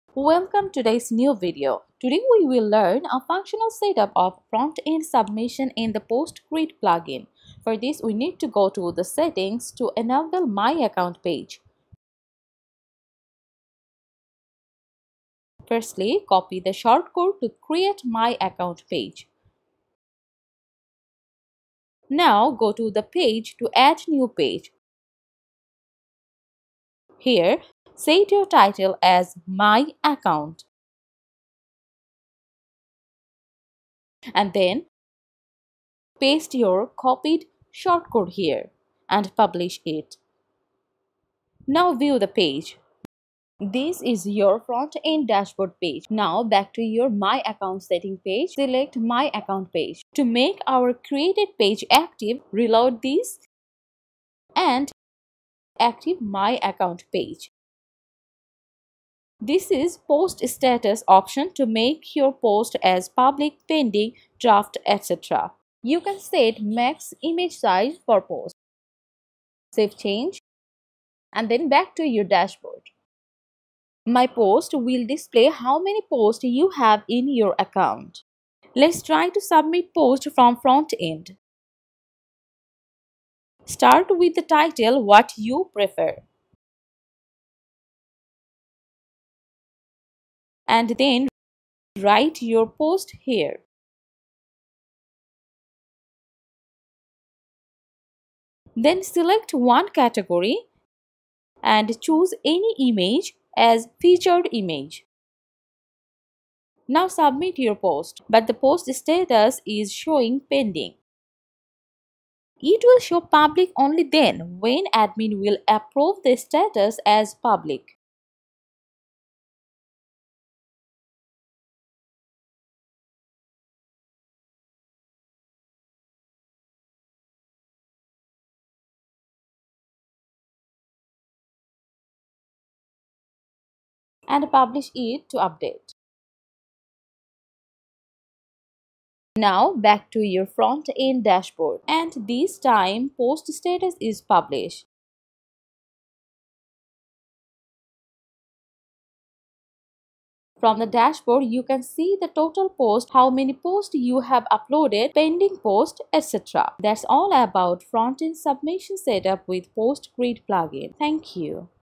How to Setup Frontend Submission in The Post Grid Plugin
Take your WordPress website to the next level! This video tutorial dives deep into setting up frontend submission functionality with The Post Grid plugin.  Allow your visitors to directly contribute content, creating a more engaging and dynamic user experience.

Get the Post Grid Plugin
Free version: